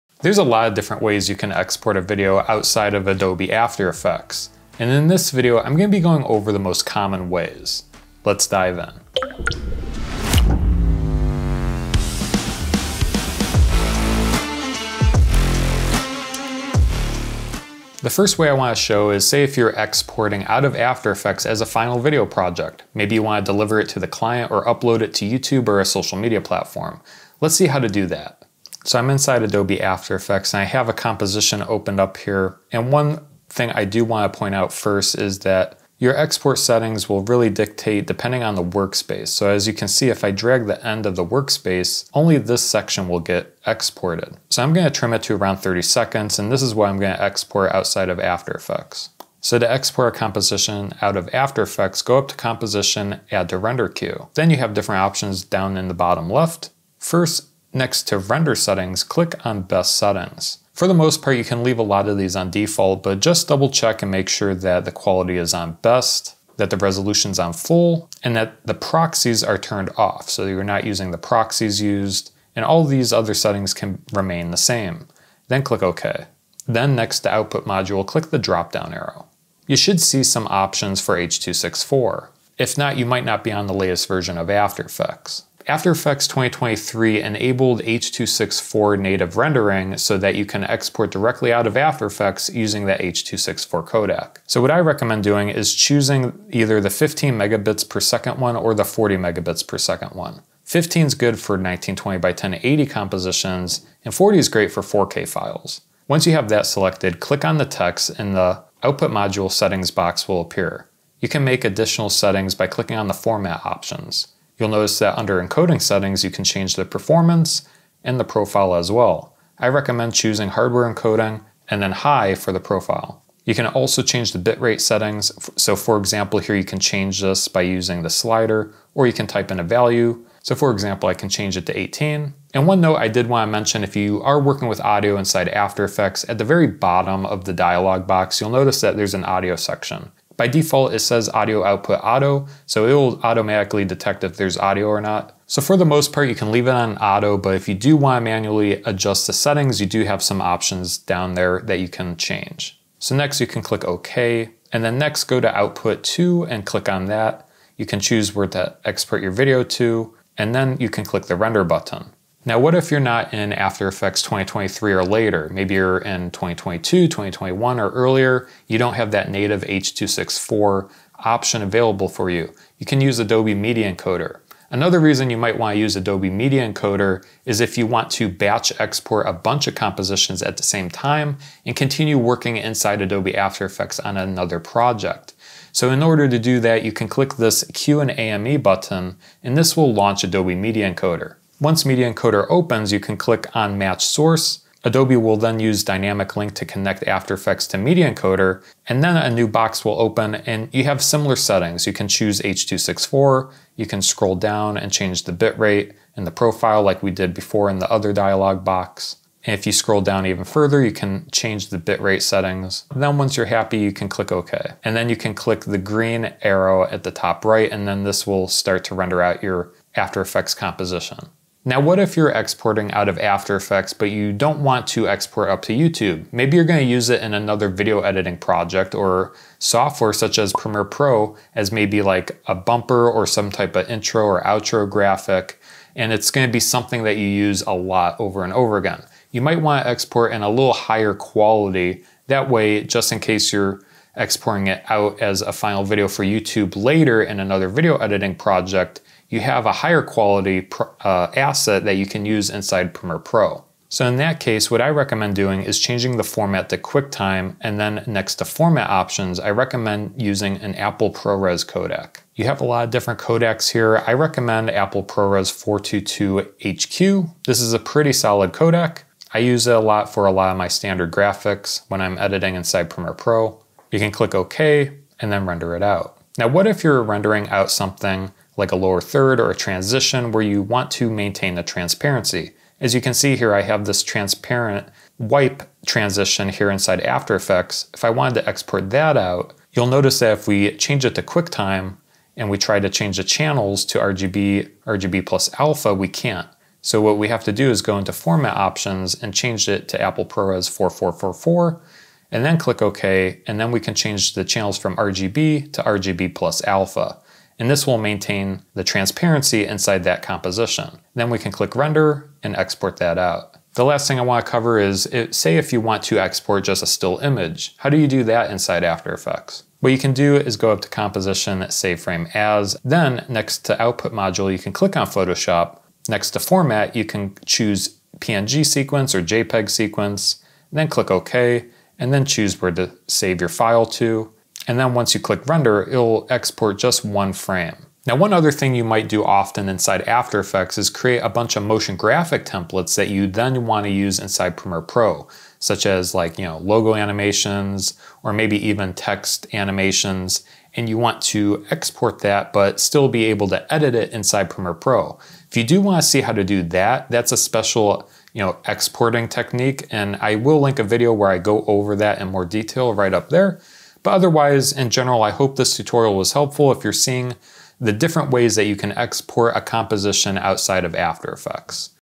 BEST Export Settings In After Effects 2024 - Complete Guide
My gear & essential filmmaking tools! 📸

ABOUT VIDEO:
There's a lot of different ways you can export a video outside of Adobe After Effects. And in this video, I'm going to be going over the most common ways. I'll first go over basic H.264 export settings for social media and then I'll go into how to batch export inside Adobe Media Encoder. I'll also go over how to export compositions as high quality asset files that you can later use in other editing softwares such as Adobe Premiere Pro. Learn how to export with transparency in After Effects, and finally I show how to export just a still frame from your After Effects timeline.

CHAPTERS:
0:00 After Effects Common Export Settings
0:16 Export After Effects For Social Media / Final Delivery
2:42 Exporting Inside Adobe Media Encoder
3:41 Export Out Of After Effects As High Quality
4:36 Exporting With Transparency In After Effects
5:13 Export A Still Frame From After Effects
5:37 Export A Motion Graphics Template In After Effects